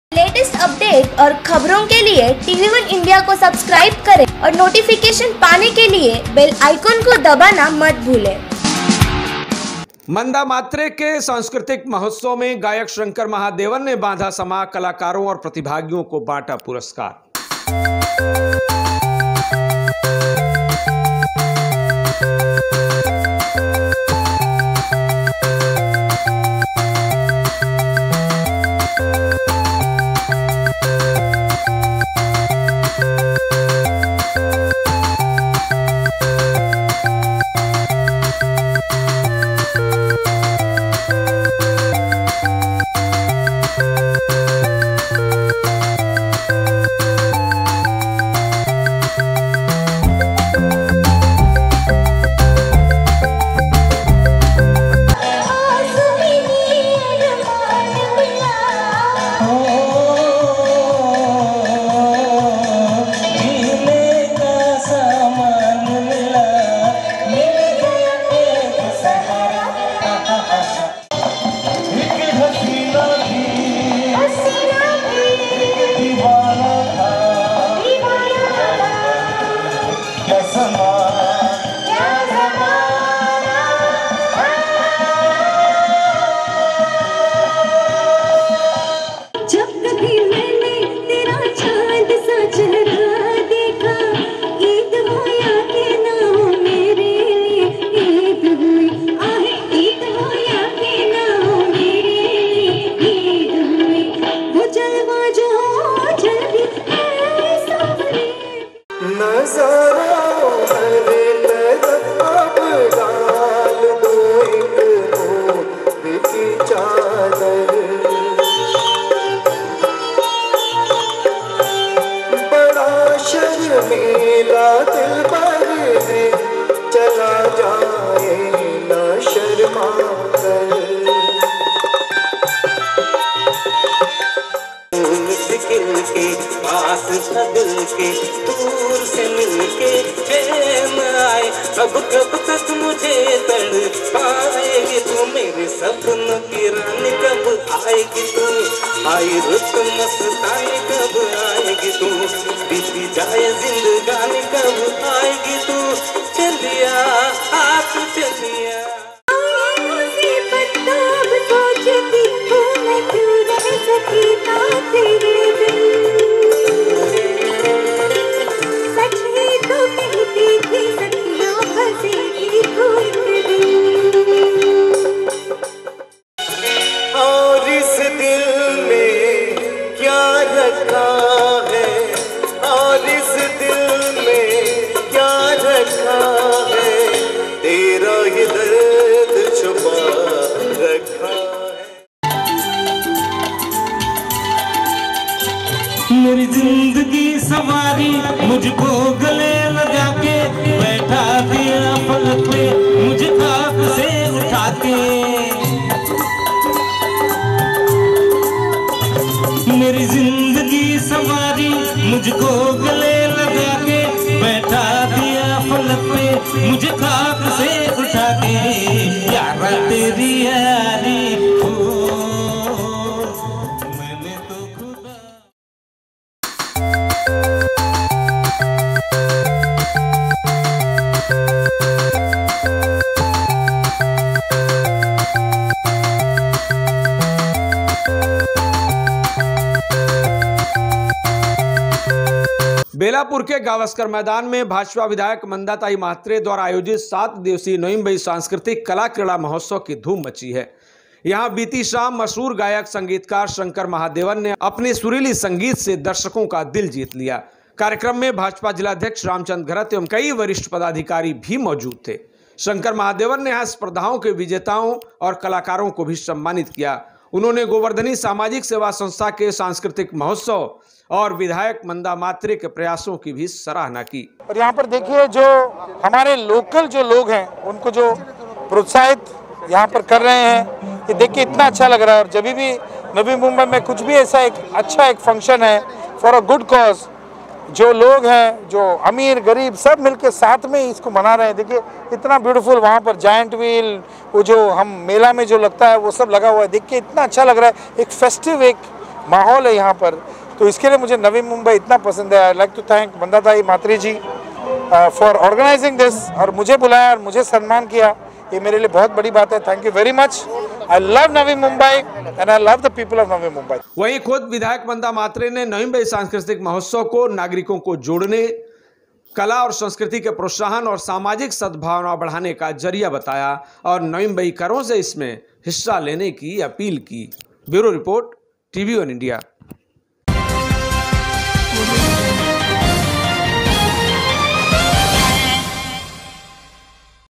BJP MLA Manda Mhatre के सांस्कृतिक महोत्सव की धूम | गायक शंकर महादेवन में बांधा समां
बेलापुर के गावस्कर मैदान में भाजपा विधायक मंदाताई म्हात्रे द्वारा आयोजित 7 दिवसीय नवी मुंबई सांस्कृतिक महोत्सव की धूम मची है. यहां बीती शाम मशहूर गायक संगीतकार शंकर महादेवन ने अपने सुरीले संगीत से दर्शकों का दिल जीत लिया. कार्यक्रम में भाजपा जिलाध्यक्ष रामचंद्र घरत एवं कई वरिष्ठ पदाधिकारी भी मौजूद थे. शंकर महादेवन ने यहां स्पर्धाओं के विजेताओं और कलाकारों को भी सम्मानित किया. उन्होंने गोवर्धनी सामाजिक सेवा संस्था के सांस्कृतिक महोत्सव और विधायक मंदा म्हात्रे के प्रयासों की सराहना भी की.